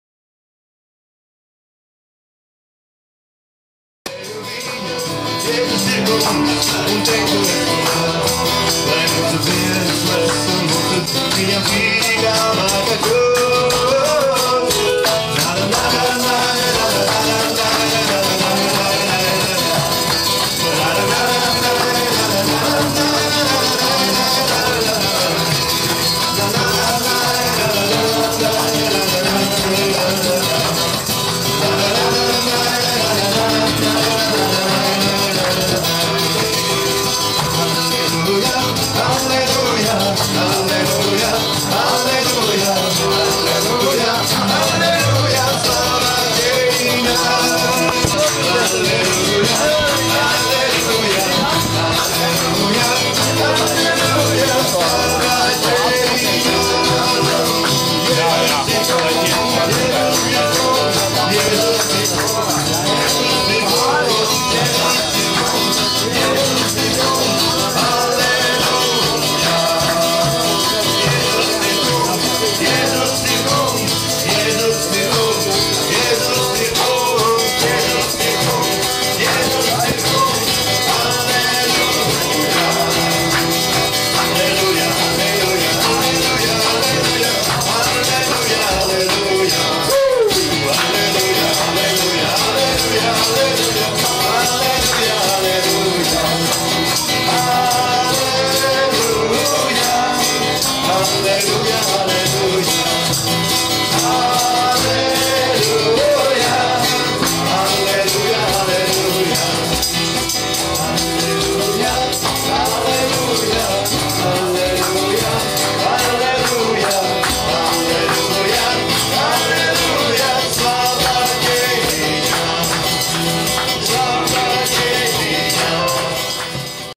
Suntaži- vīru nometne 2012.mp4
Kristīga vīru nometne kopā ar misionāru Donu Tērbitu. 2012.g. 24.-26. aug.